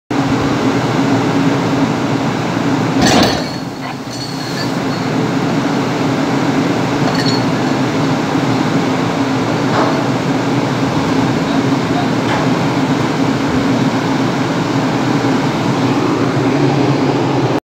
Test of the lifeboat
5-year load test of the lifeboat and on-load release mechanism.